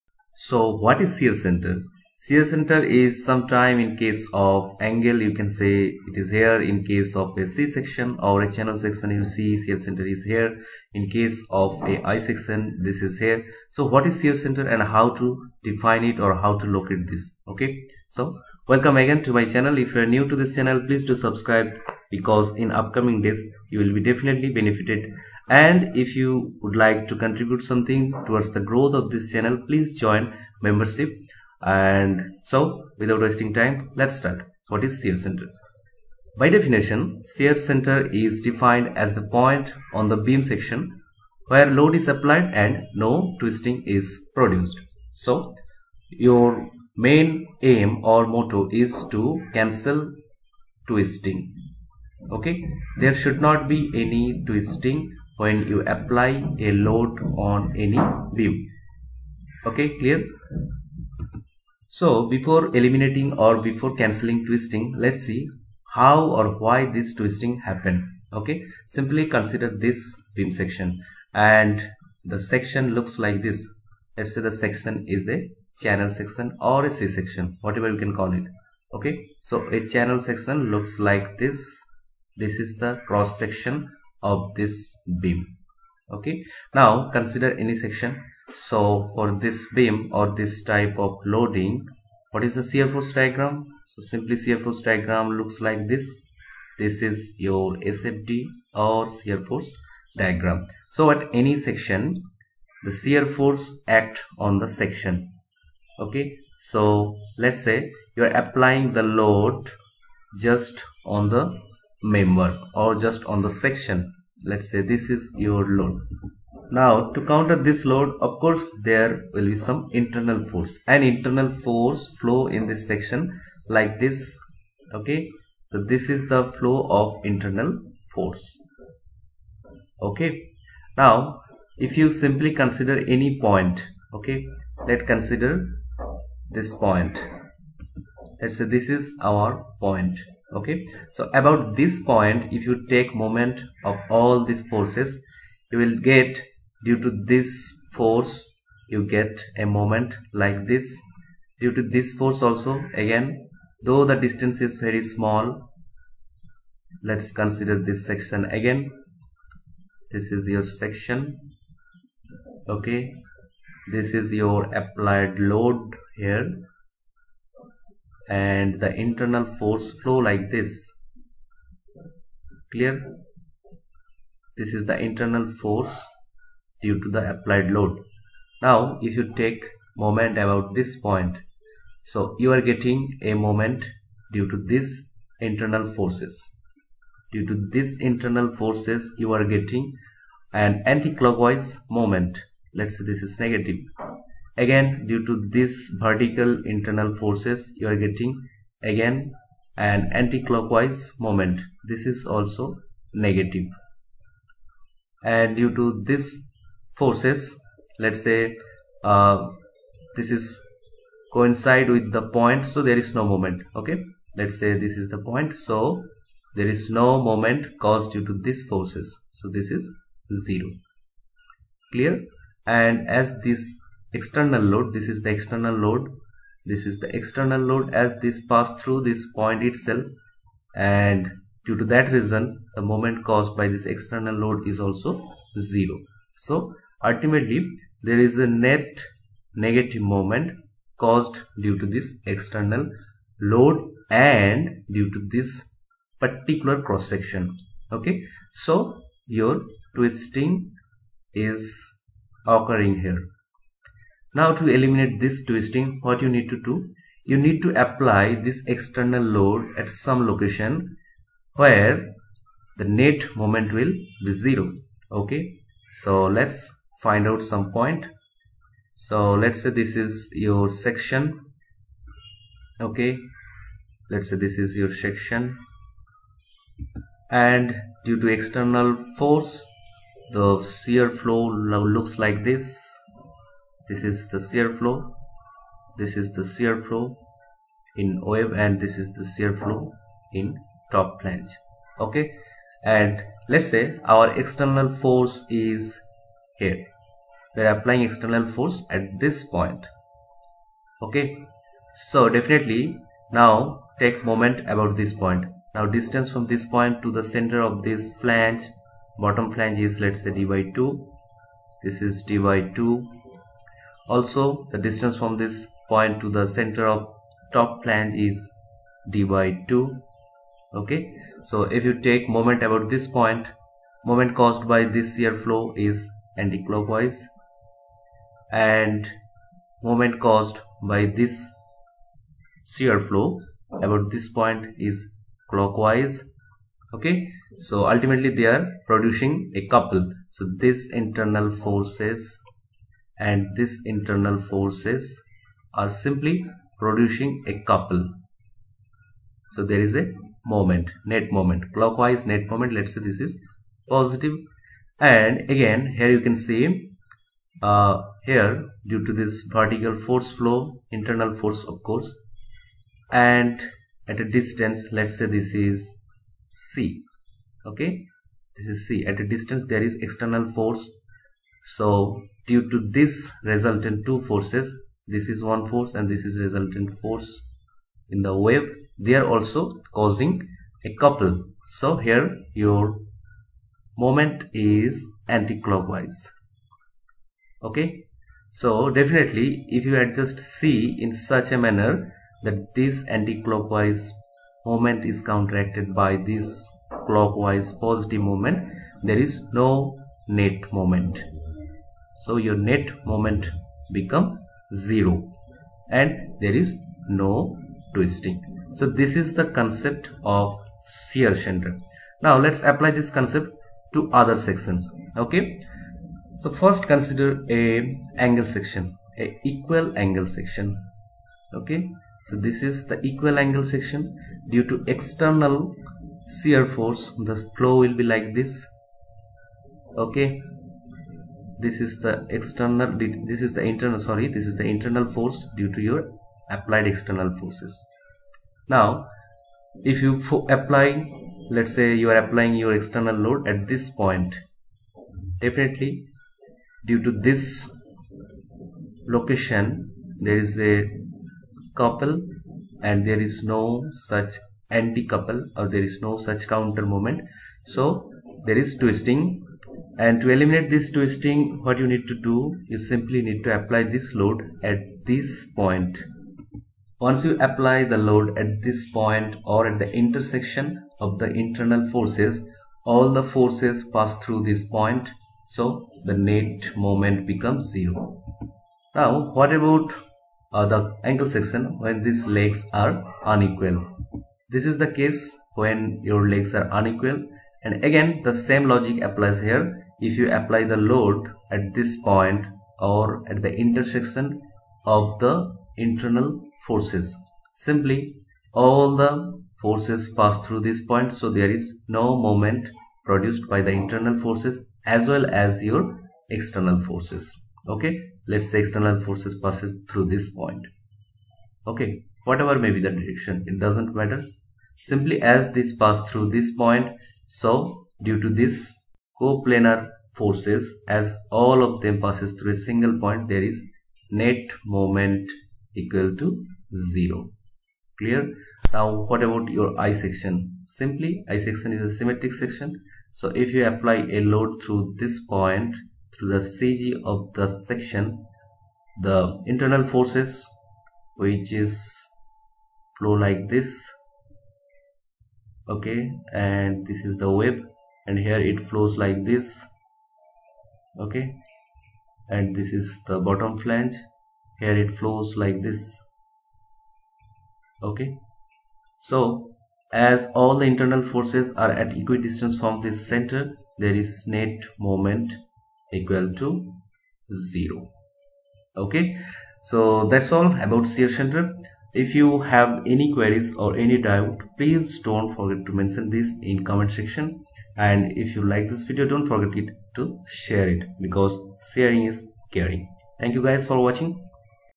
Shear Center : Concept Behind Shear Center and Application
For Unsymmetrical section, during Bending Twisting is a major problem.
To overcome twisting problem, the concept of shear center has come into picture.
Watch this video to know the basic behind Shear Center.